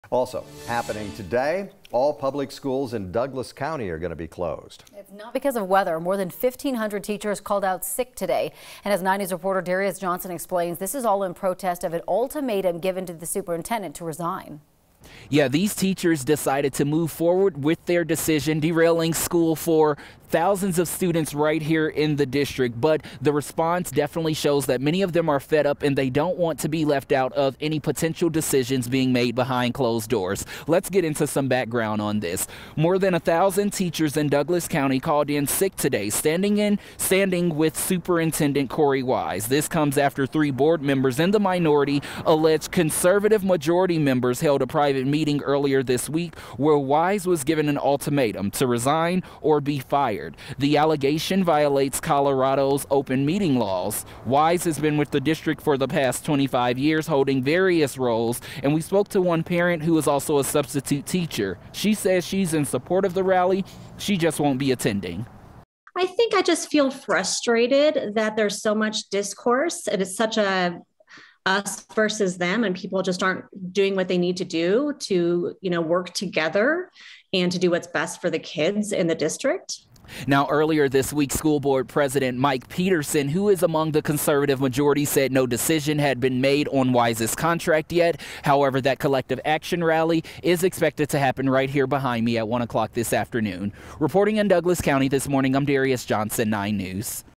Douglas County School District cancels class Thursday after teachers call out sick
The Douglas County School District has canceled classes on Thursday because more than 1,000 teachers called out sick in protest.

9NEWS (KUSA) is located in Denver, Colorado.